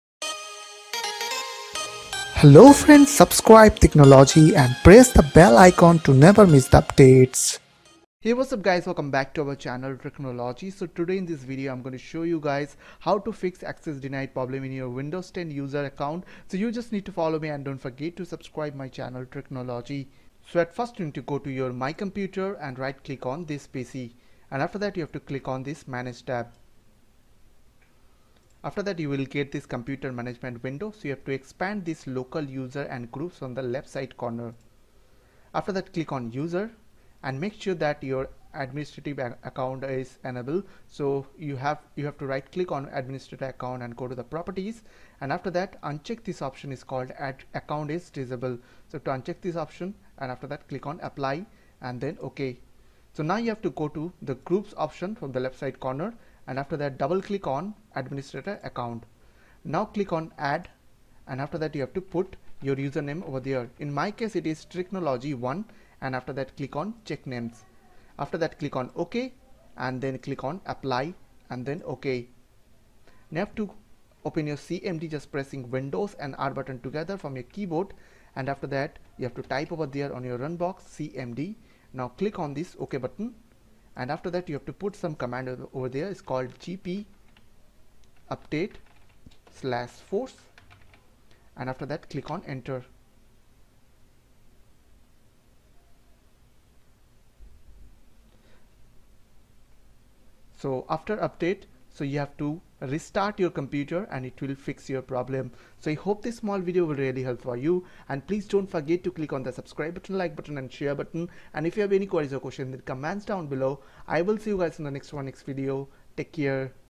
How to Fix “Access is denied” Windows 10 error
In this video I will show you guys how to fix windows 10 access is denied problem.

here is the command for force update grouppolicy : gpupdate /force

1. Mic
2. Microphone Stand
3. Headphone
4. Phone
5. Strip LED Light
6. Camera

Windows10 # Fix“Accessisdenied”